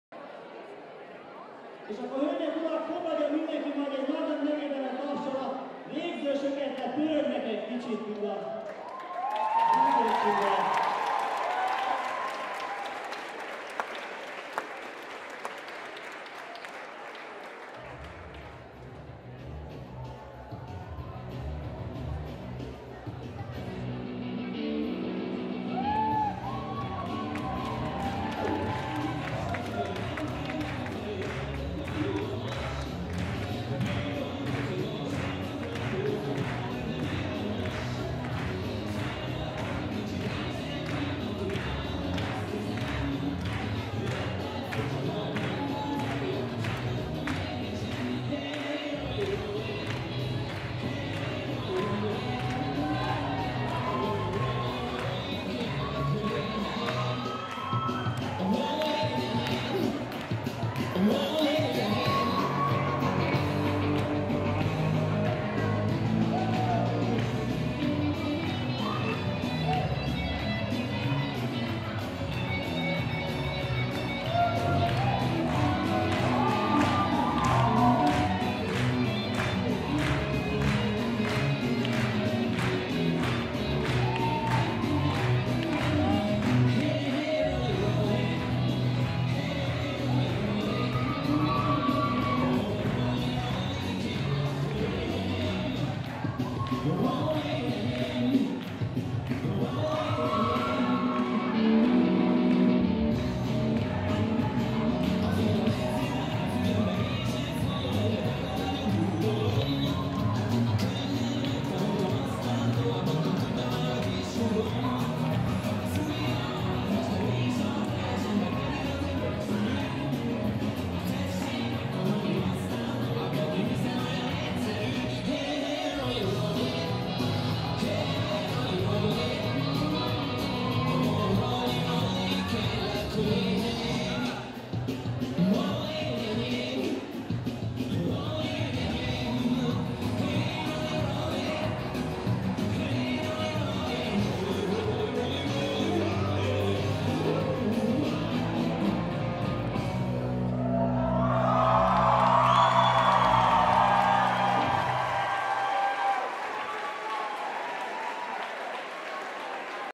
Lovassy bal rocky
A 2020. február 8-án megrendezett Lovassy-bál rockyjának videója.
A felvételt Cziráki Péter készítette.